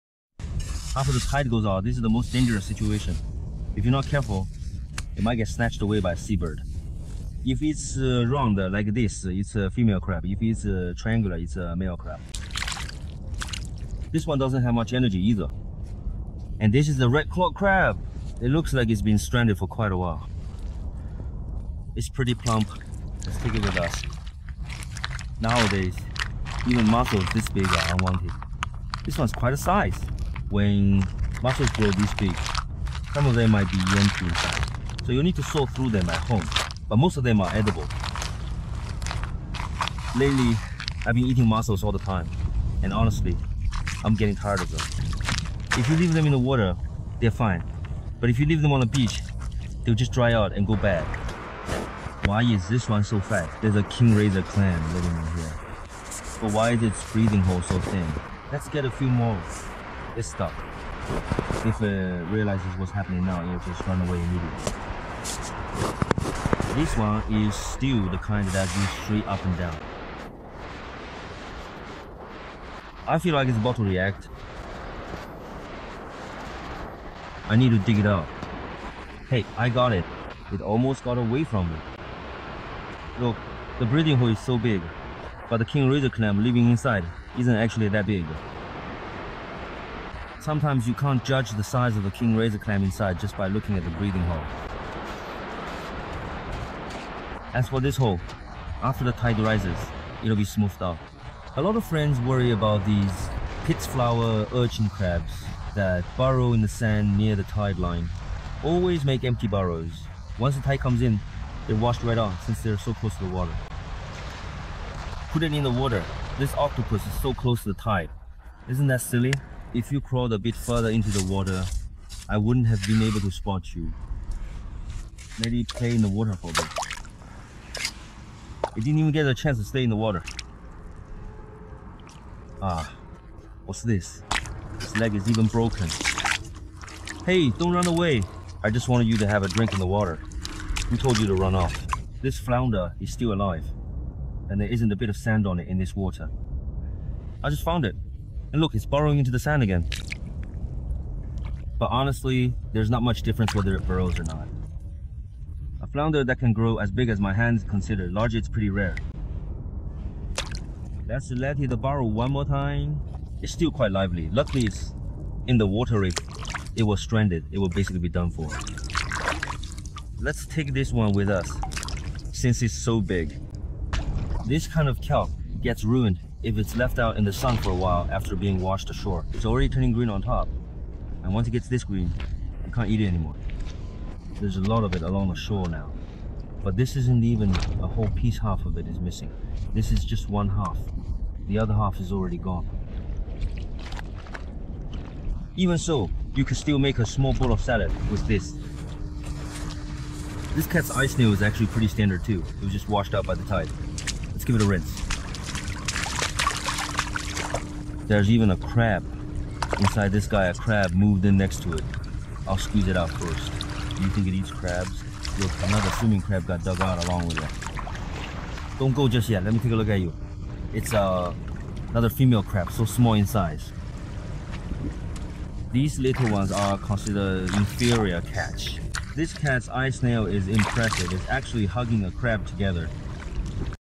Digging Up the Strangest Sea Creatures!
Uncover the hidden world of cat's eye snails! Watch as these stunning mollusks blend into the sand, revealing their iridescent shells and mysterious behavior 🌊🐚